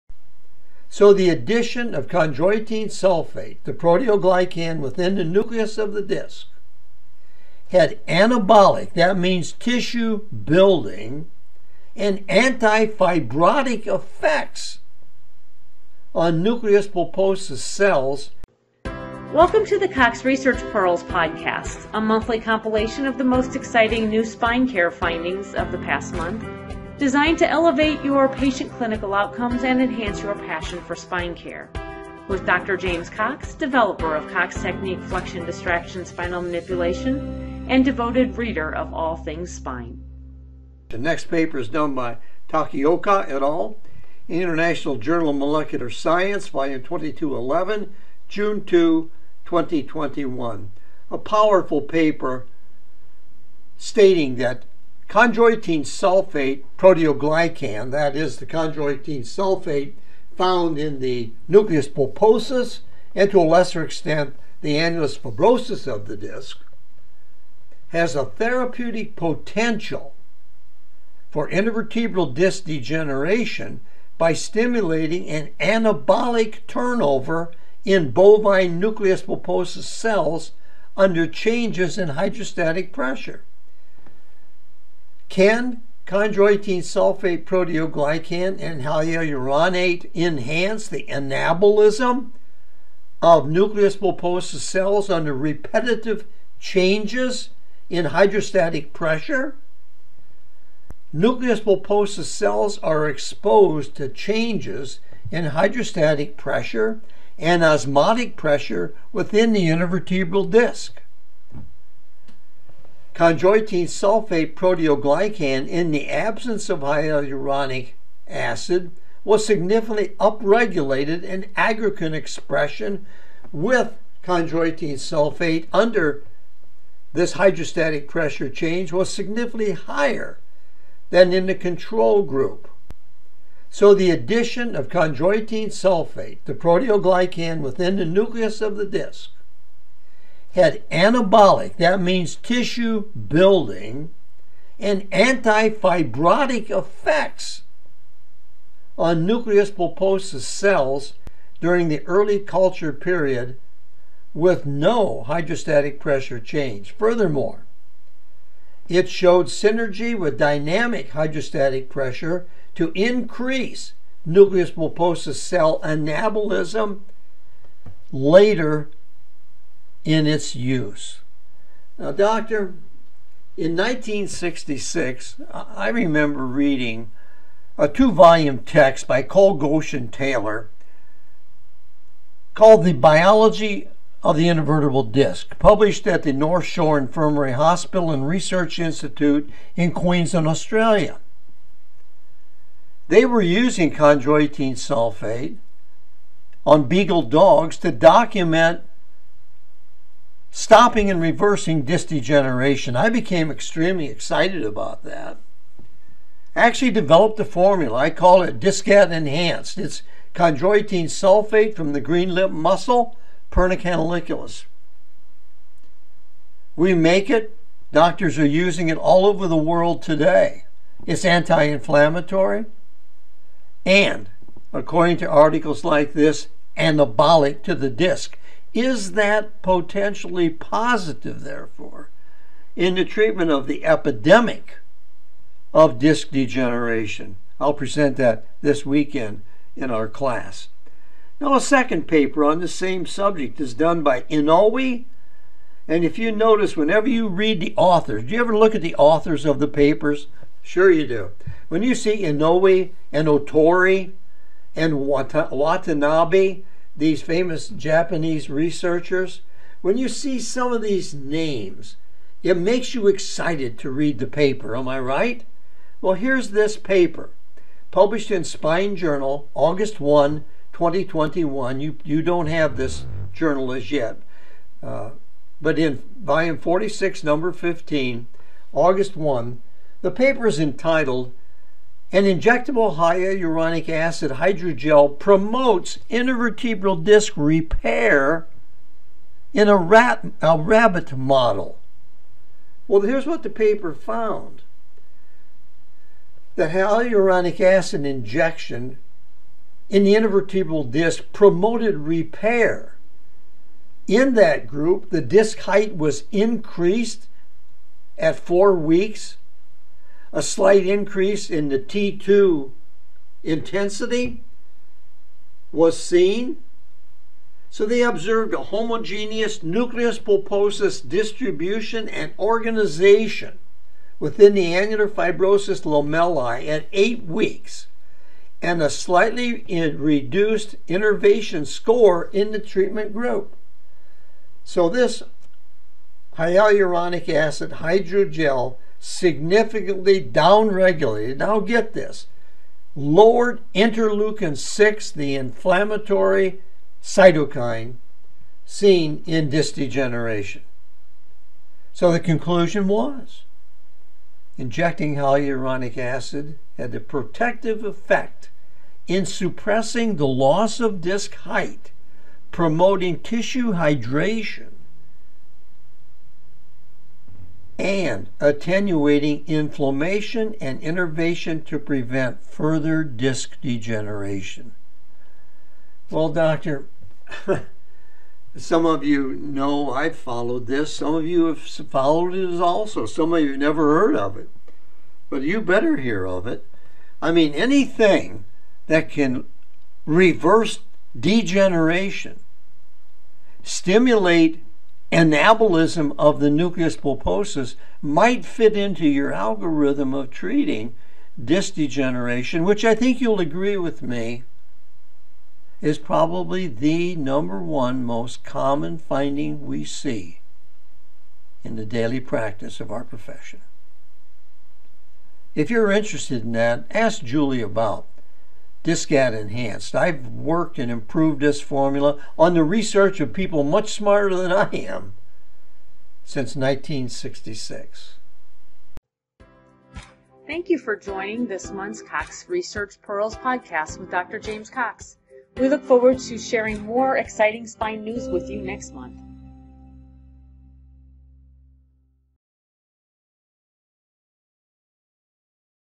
Cox® Research Pearl: Chondroitin Sulfate and Disc Degeneration (takeoka/inoue research)
Listen as Dr. James Cox presents the recent research from Takeoka and Inoue about disc degeneration and potential regeneration - chondroitin sulfate, conditions optimal for regeneration, and more.